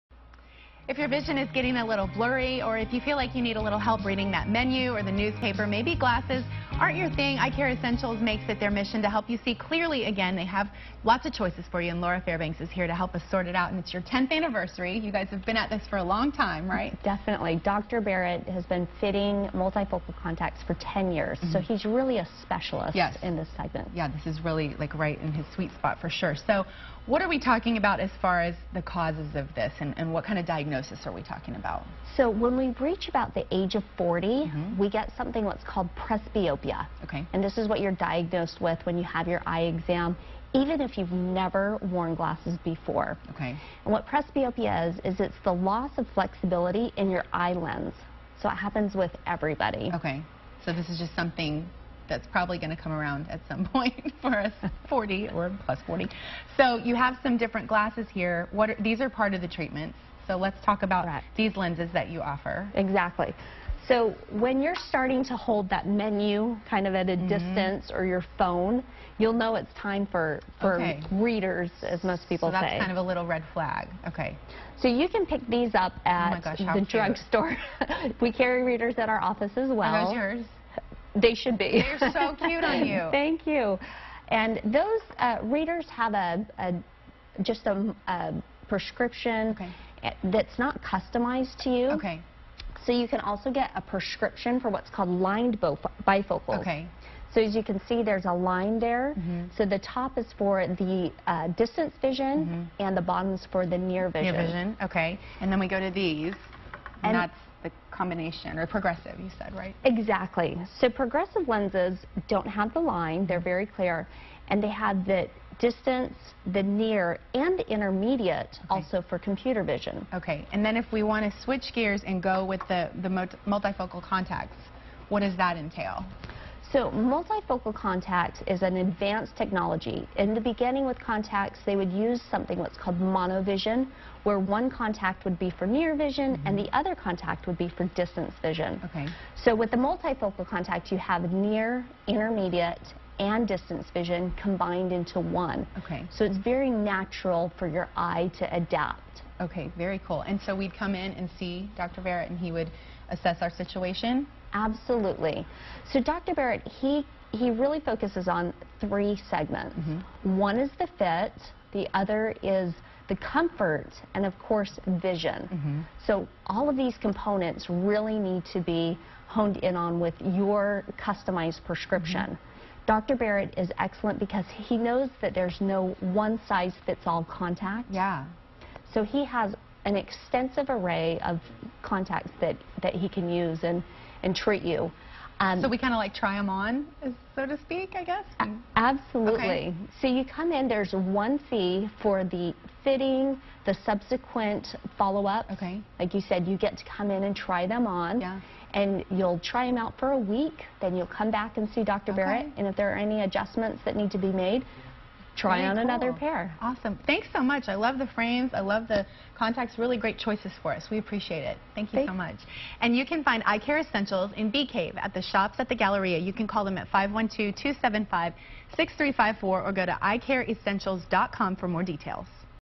Advanced Technology for Vision After Age 40
Featuring:
Hosts:
Dr. Nolan Barit, Optometrist Eyecare Essentials LinkedIn
Laura Fairbanks MBA, Eyecare Essentials, Medical & Small Business REALTOR of KW Commerical LinkedIn

Guests:
Dr. Scott Hruby, Optometrist
Joyce Sandberg, Yoga Instructor
Jeff O'Brien, Professional Photographer
Brandon Aregger, Rig Manager for Unit Drilling Corp